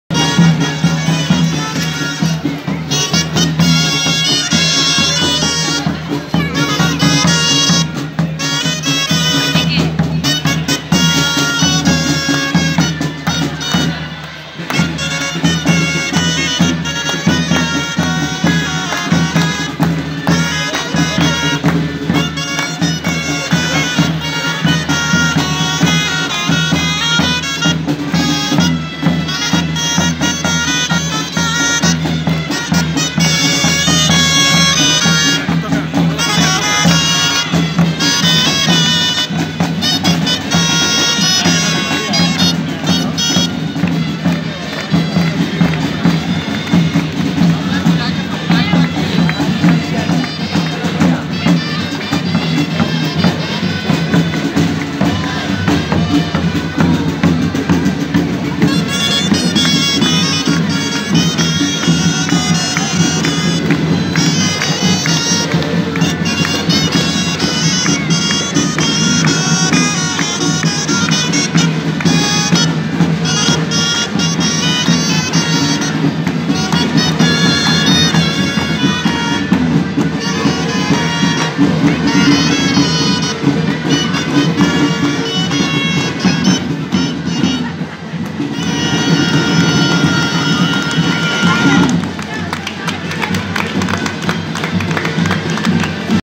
Trobada de gegants a Sant Pere de les Puel·les 2019 - Cercavila (III)
El dissabte 23 de novembre de 2019 tingué lloc la III Trobada de gegants a Sant Pere de les Puel·les, que va començar davant l'església i acabà davant del Mercat de Santa Caterina.